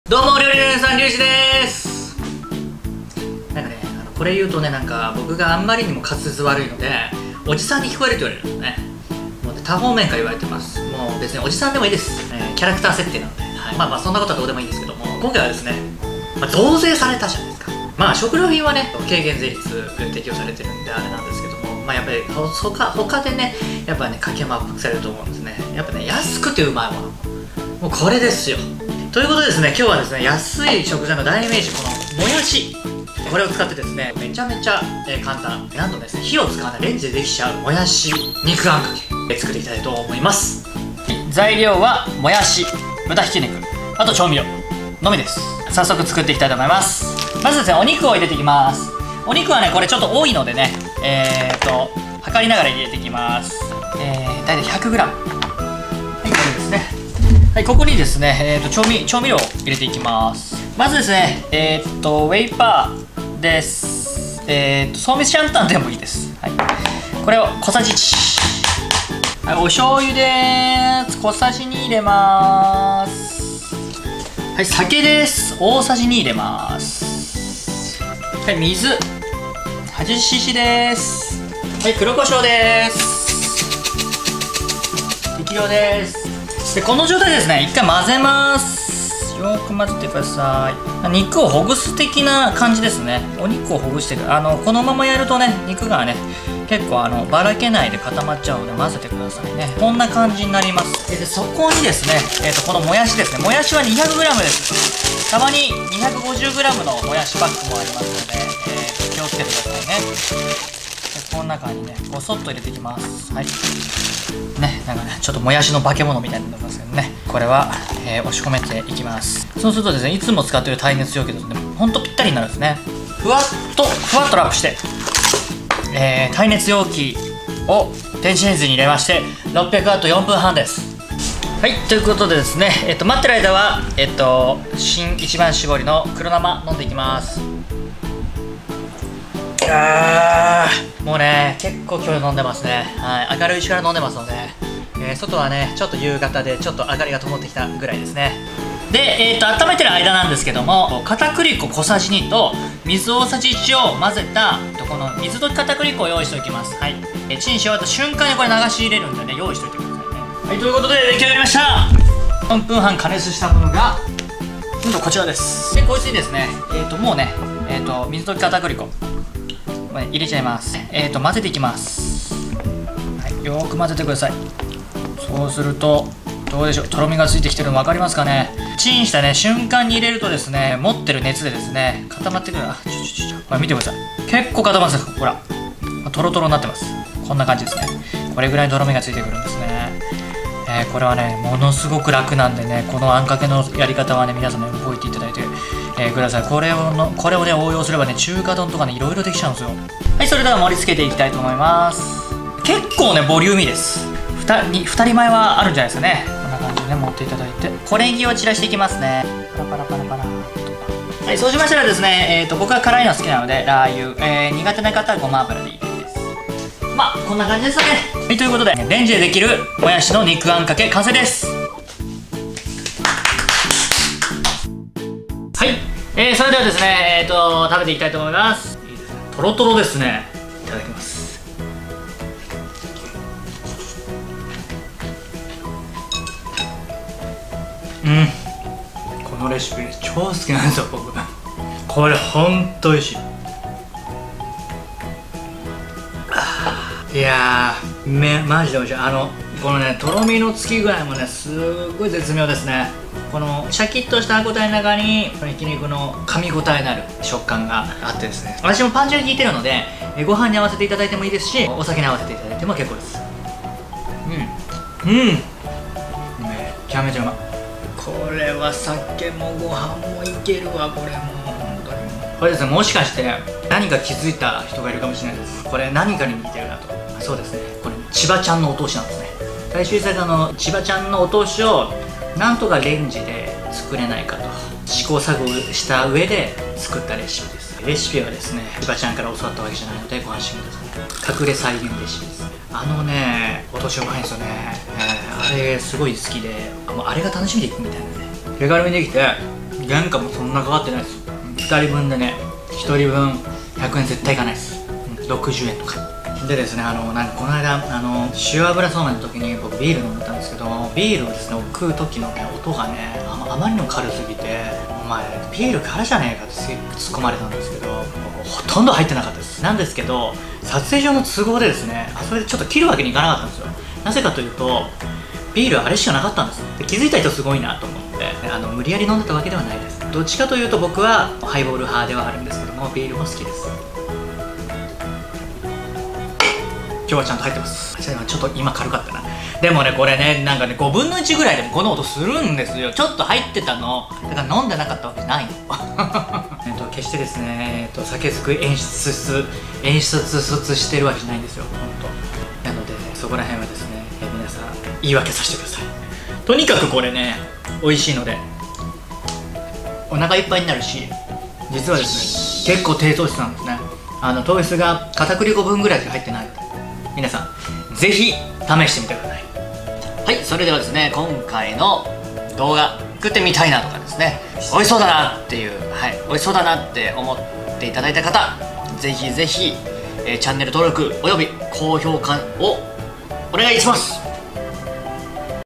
あの超有名店の激ウマお通し！？【もやし肉あんかけ】
レンジで簡単！低糖質かつ節約レシピ！超有名店【ちばチャン】のお通しを再現してみました！

★今回のレシピはこちら↓
【もやし肉あんかけ】
もやし　　　　　　　　　　　　　　２００グラム
豚ひき肉　　　　　　　　　　　　　１００グラム
ウェイパーもしくは創味シャンタン　小さじ１
水　　　　　　　　　　　　　　　　８０㏄
黒胡椒　　　　　　　　　　　　　　適量
◇とろみをつけるための水溶き片栗粉◇
★お好みで
追い黒胡椒　　　　　　　　　　　　適量

フリーBGM
DOVA-SYNDROME